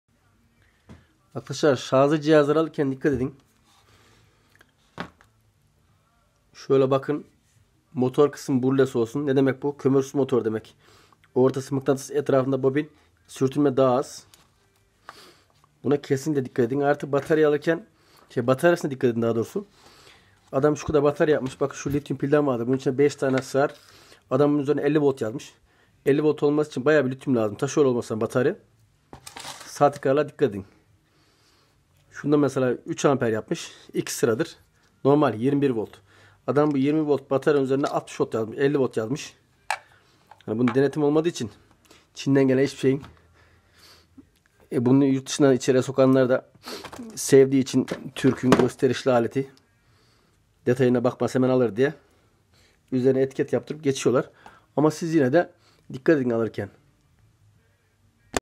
şarjlı cihazları alırken dikkat edin
aldığınız cihazların motoru kömürsüz motor olsun ayrıca havalandırmadan içine bakın öylemi diye kömürsüz motor diye fırçalı motorlu aleti satan ahlaksız çok artı batarya ya dikkat edin üzerinde yazan değer ile verdiği voltaj aynımı diye denetim olmadığı için çok sahtekarlık dönüyor bu işte